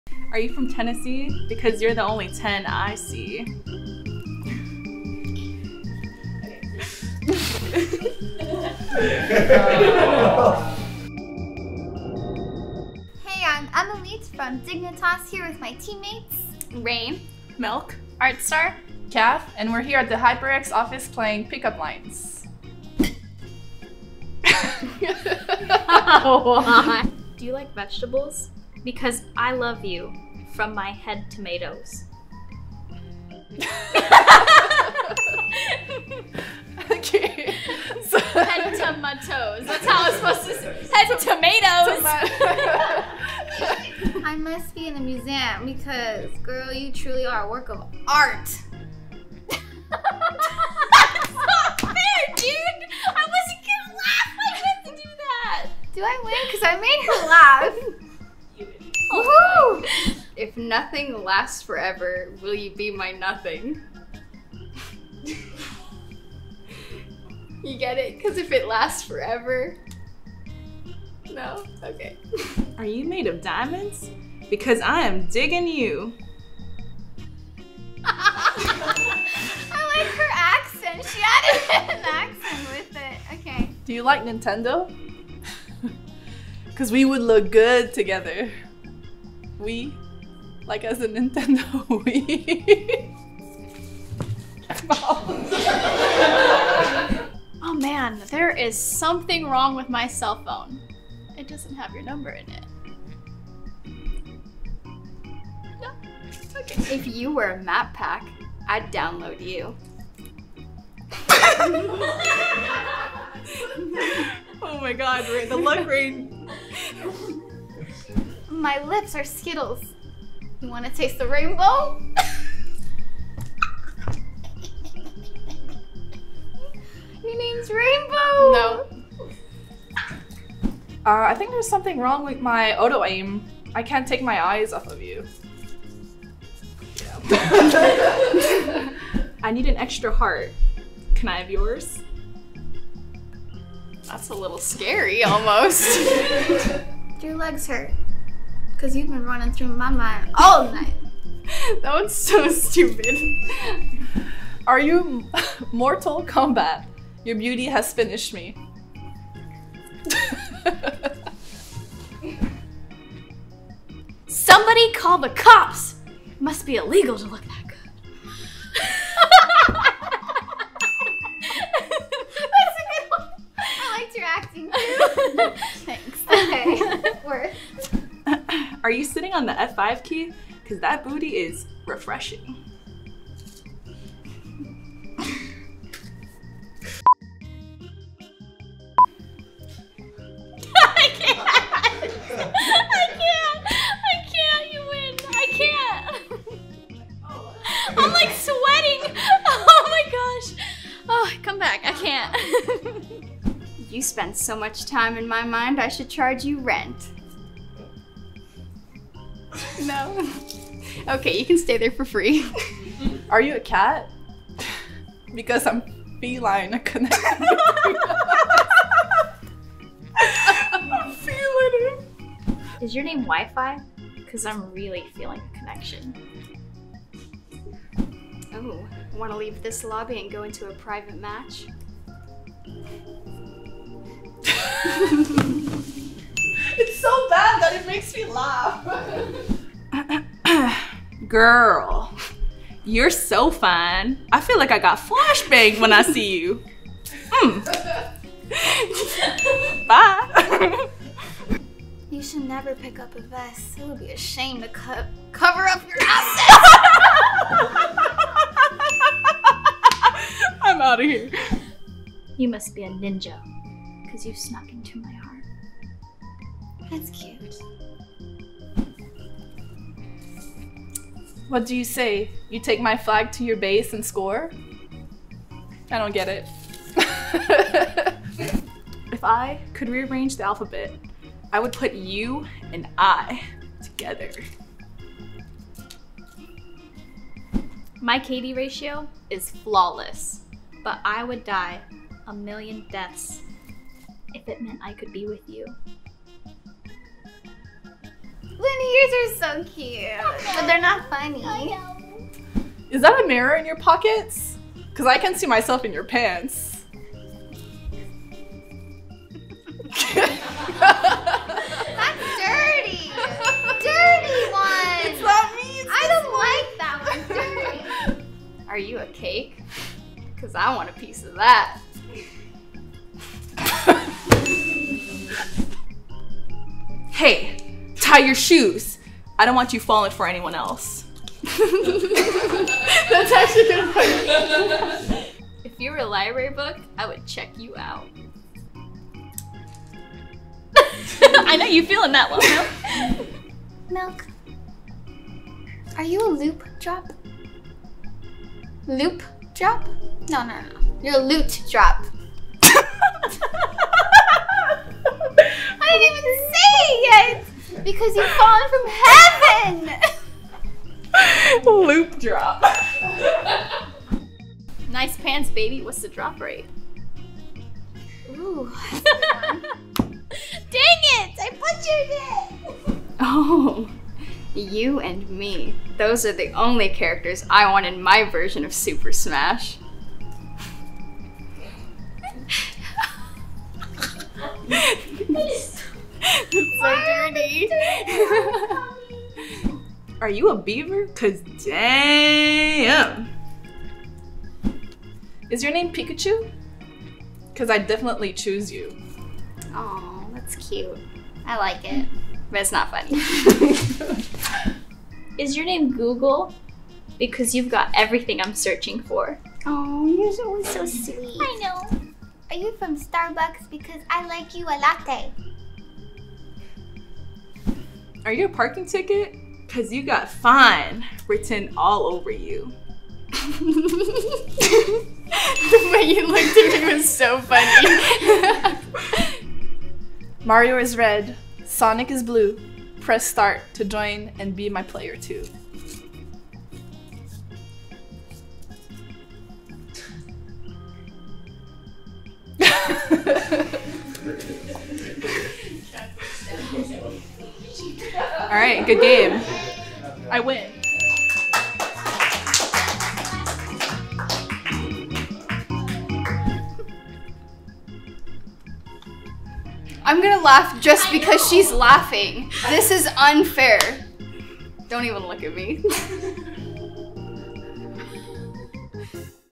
Dignitas Female CS:GO – Pickup Line Challenge
The Dignitas Female CS:GO team swings by the HyperX office to deliver some of their best gaming related pickup lines for the Pickup Line Challenge. If the smooth operator delivering the line can make their target laugh, they earn a point, if their line falls flat, then the next person steps up.